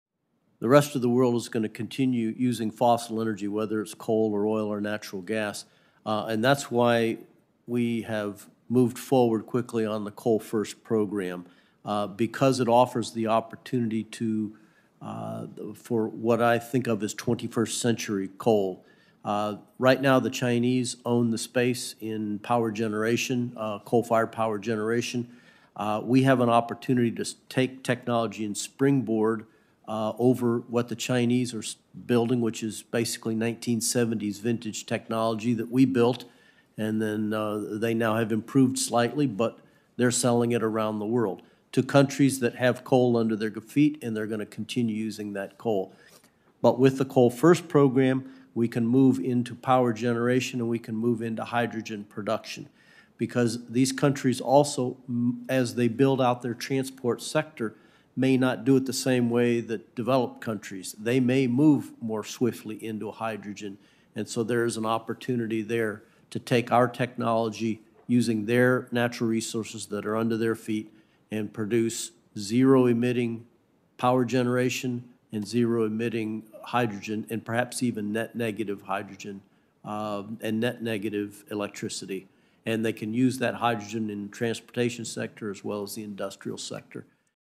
Former Secretary at the U.S. Department of Energy promotes "coal first program"
Steven Winberg, Former Assistant Secretary for Fossil Energy at the U.S. Department of Energy, testifies at the "Full Committee Hearing to Examine Development and Deployment of Large-Scale Carbon Dioxide Management Technologies" held by the Senate Committee on Energy and Natural Resources on July 28, 2020.

This clip is featured in our full episode on the Inflation Reduction Act